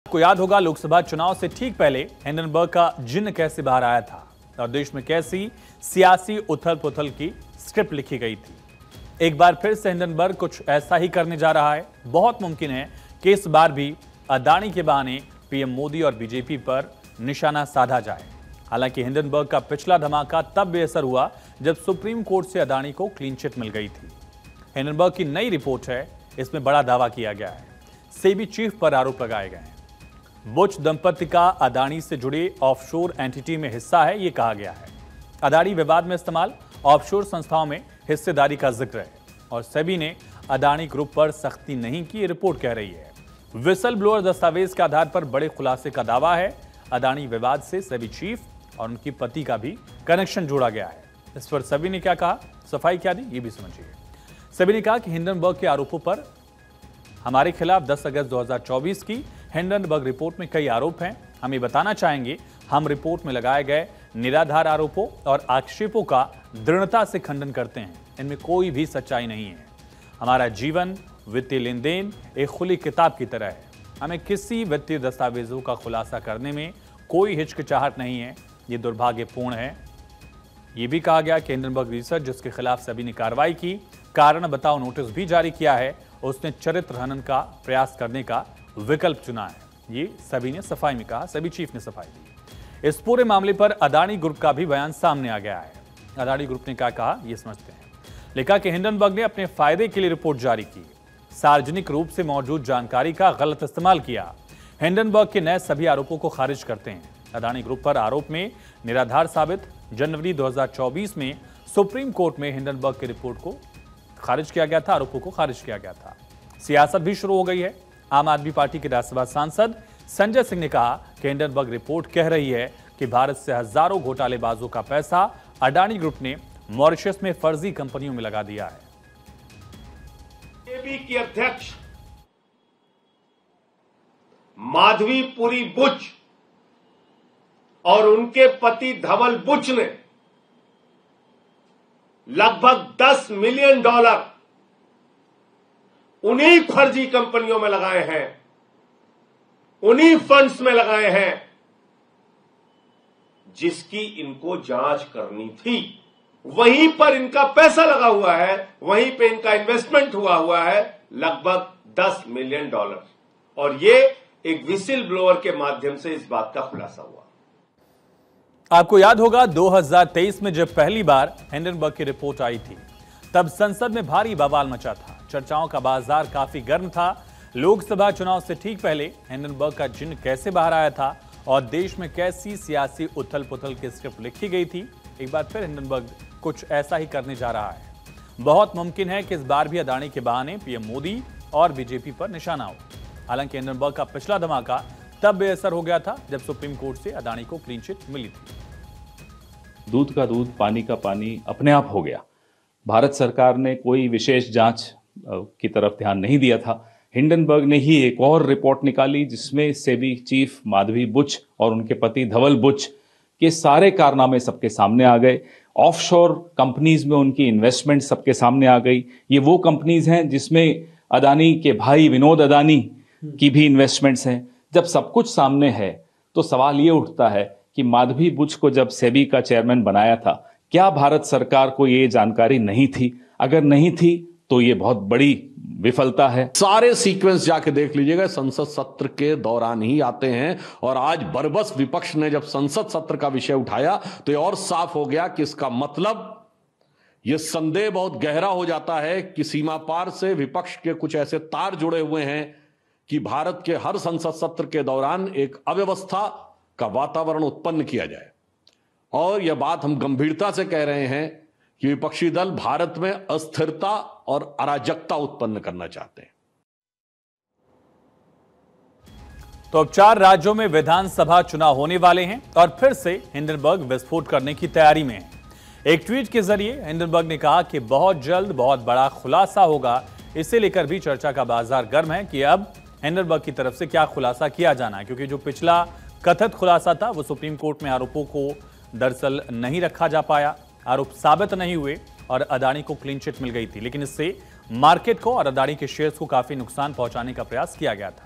फिर बाहर आया हिंडनबर्ग का 'जिन्न' | Breaking News | Adani Group | India | Bharat 24
आपको याद होगा लोकसभा चुनाव से ठीक पहले हिंडन बर्ग का जिन्न कैसे बाहर आया था और देश में कैसी सियासी उथलपुथल की स्क्रिप्ट लिखी गई थी. एक बार फिर हिंडनबर्ग कुछ ऐसा ही करने जा रहा है बहुत मुमकीन है कि इस बार भी अदाणी के बहाने पीएम मोदी और बीजेपी पर निशाने पर होगी हालांकि हिंडनबर्ग का पिछला धमाका तब बेअसर हो गया था जब सुप्रीम कोर्ट से अदाणी को क्लीनचीट मिल गई थी...

You must remember how the genie of Hindenburg came out just before the Lok Sabha elections and how the script of political turmoil was written in the country… Once again Hindenburg is going to do something similar. It is very possible that this time too, PM Modi and BJP will be targeted on the pretext of Adani, although Hindenburg's previous blast became ineffective when Adani got a clean chit from the Supreme Court…

Anchor Name : Ayush Sooryavanshi  | Show Name : News | Time : 09: 30 PM | Date : 11/08/2024 |